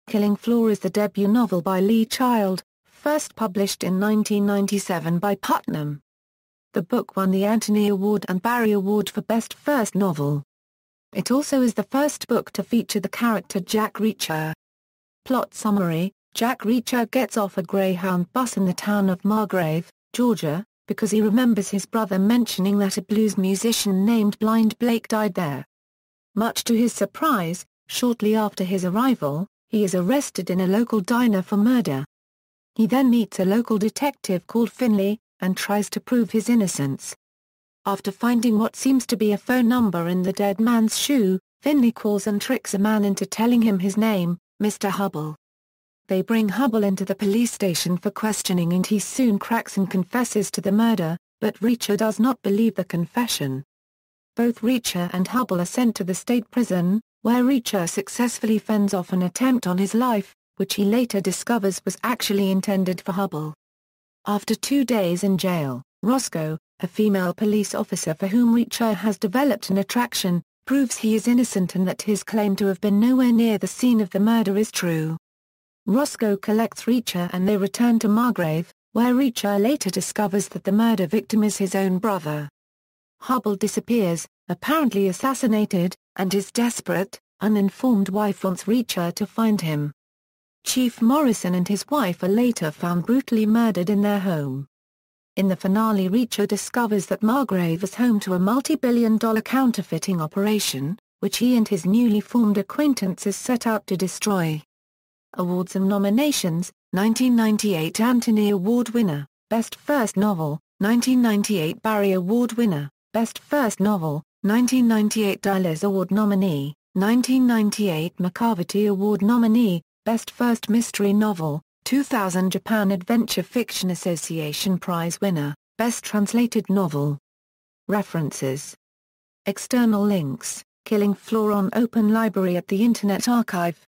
Killing Floor (novel)
Killing Floor is the debut novel by Lee Child, first published in 1997 by Putnam. The book won the Anthony Award and Barry Award for best first novel. It also is the first book to feature the character Jack Reacher.